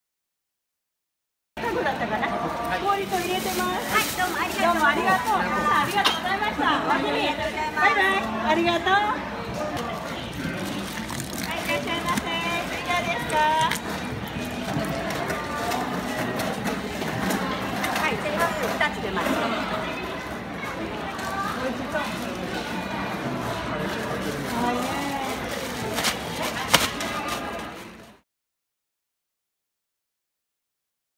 This is What Happens When You Buy a Live Octopus at a Market in Japan
The poor thing has its beak ripped out and is then stuffed into a body bag.